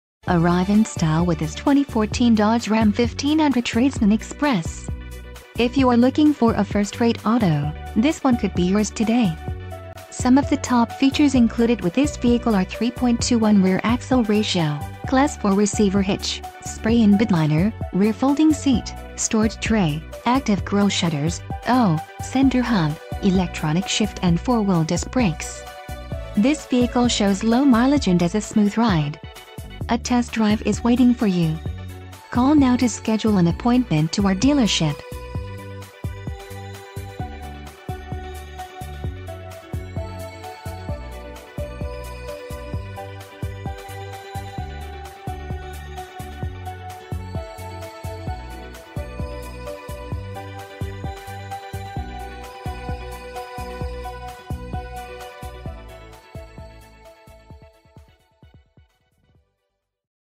2014 Dodge Ram 1500 Tradesman/Express - Sulphur Springs D...
2014 Dodge Ram 1500 Tradesman/Express Condition: New Mileage: 3 Transmission: Automatic Exterior Color: Red Doors: 4 Stock: EG238260 Cylinders: 8